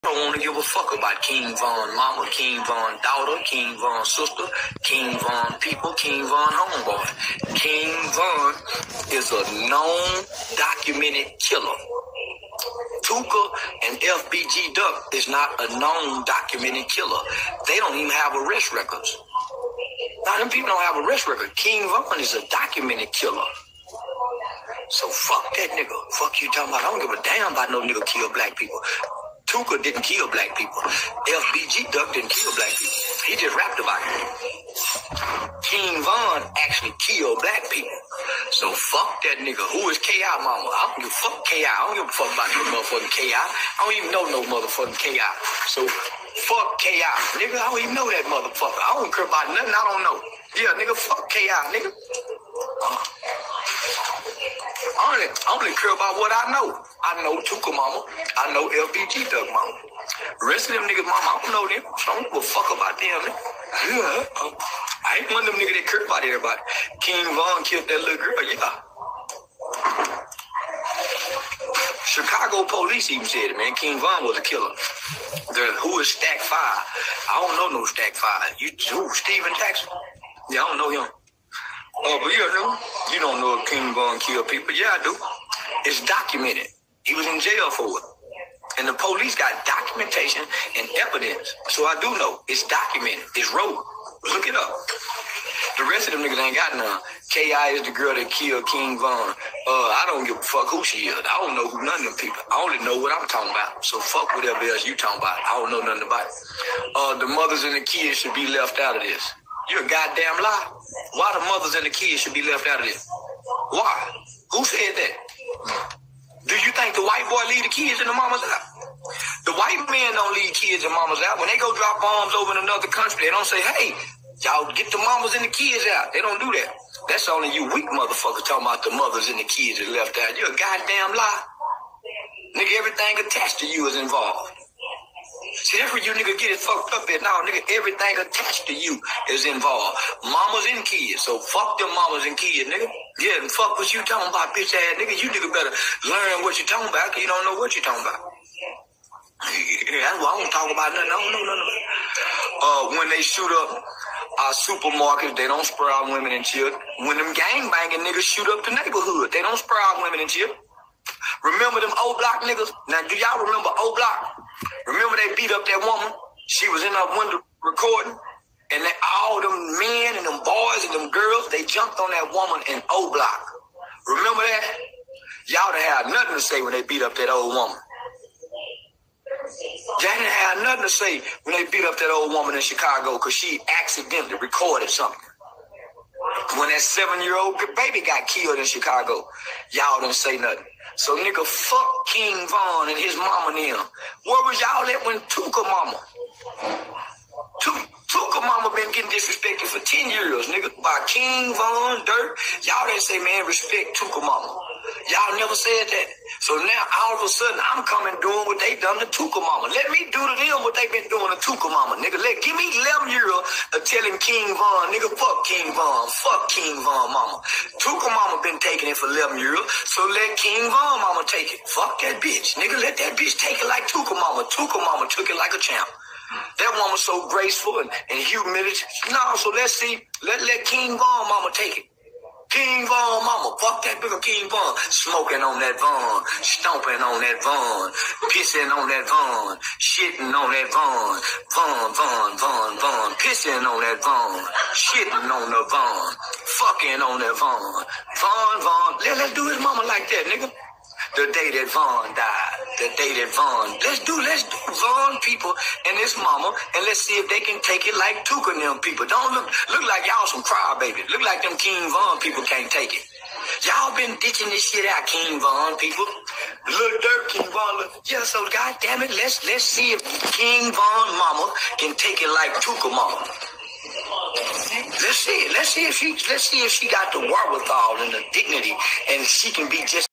Charleston white says "f*** KING VON'S MOTHER"
Charleston white let's his felling known about king von and his mother